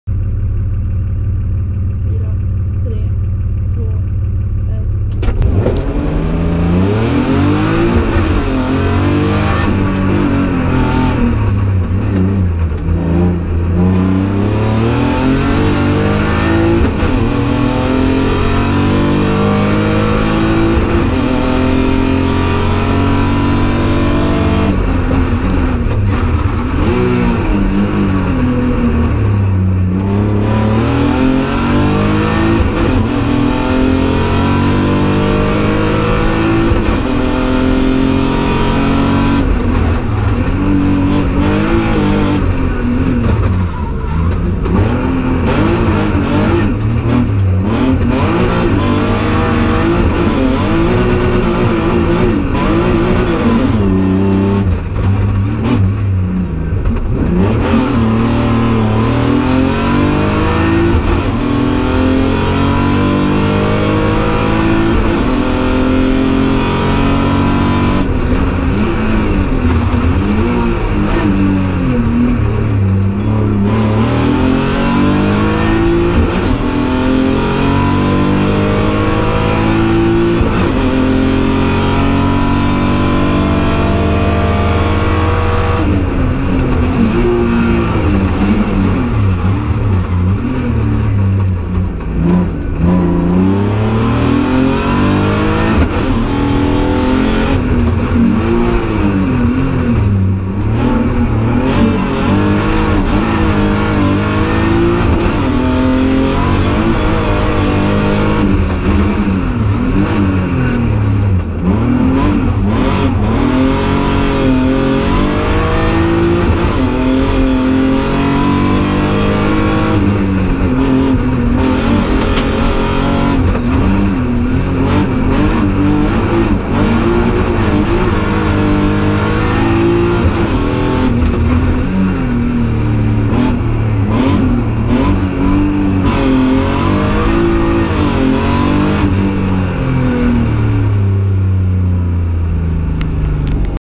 Lundasprinten 2016 3e åket
Tim Joons/Alexandra Grönberg  BMW 325 Grupp-H B-förare